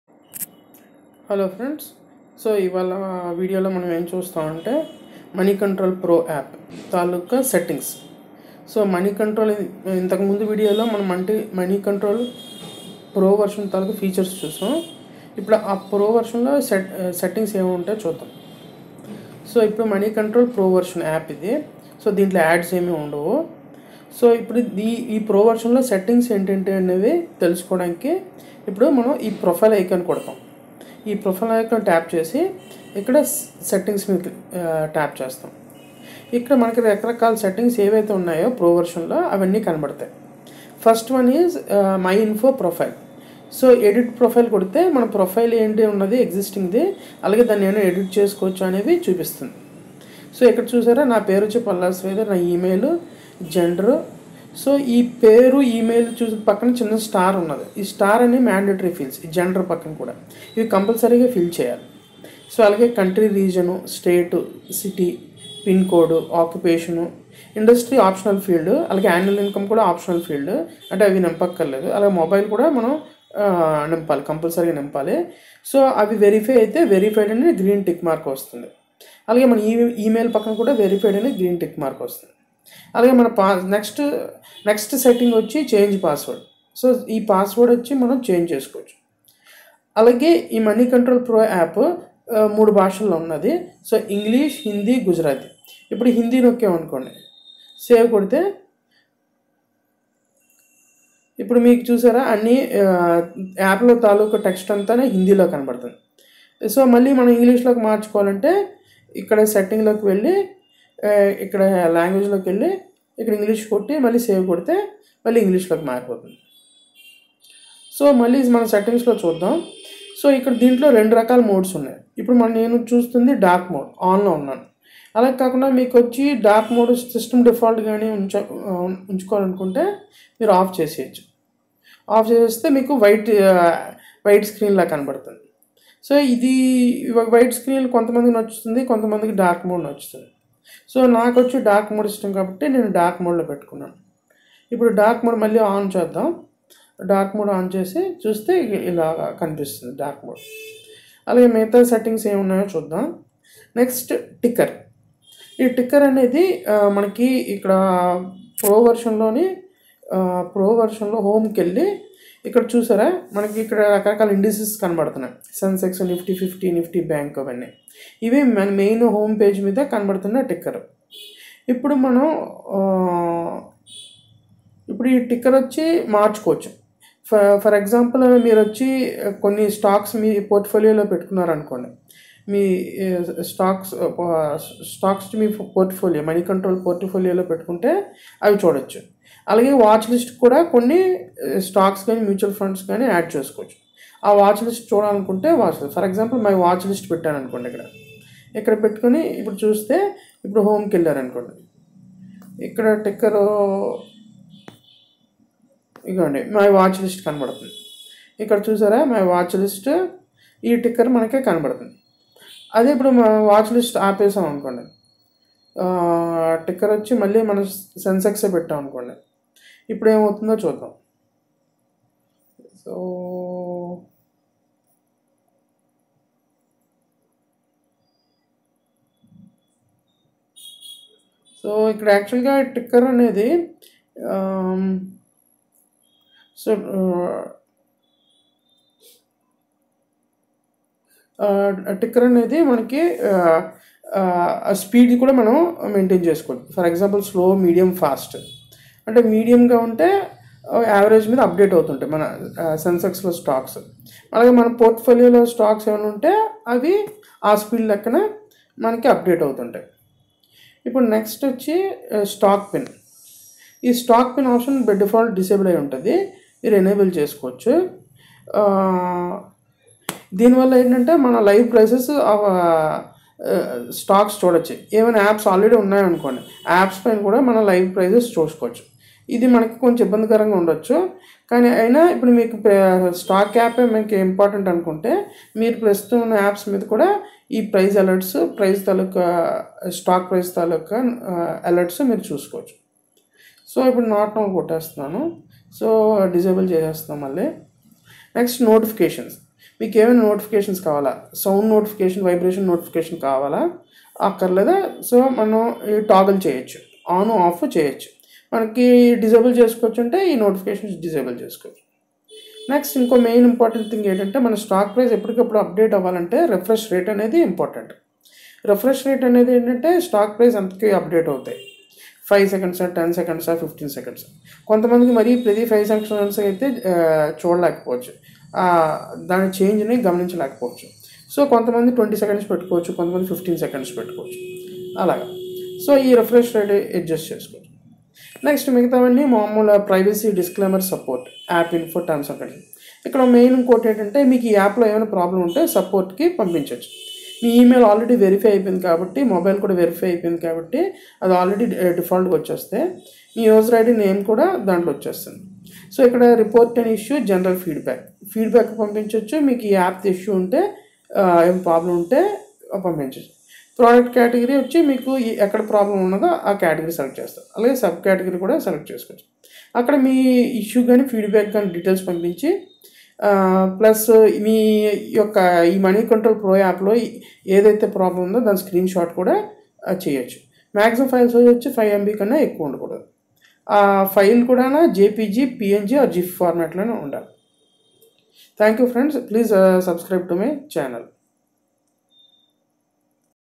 Moneycontrol Pro Settings inTelugu - Explanation of each Option
ప్రకటన రహిత అనుభవం
వేగవంతమైన యాక్సెస్ & క్యూరేటెడ్ మార్కెట్ల డేటా
లోతైన ప్రాథమిక విశ్లేషణ,
వార్తలు - క్రియాత్మక పెట్టుబడి ఆలోచనల కోసం విశ్లేషించబడ్డాయి
గ్లోబల్ ఈవెంట్స్ డిబ్రీ,
మార్కెట్ గురువులు మరియు మా సంపాదకులకు ప్రాప్యత మరియు మరెన్నో…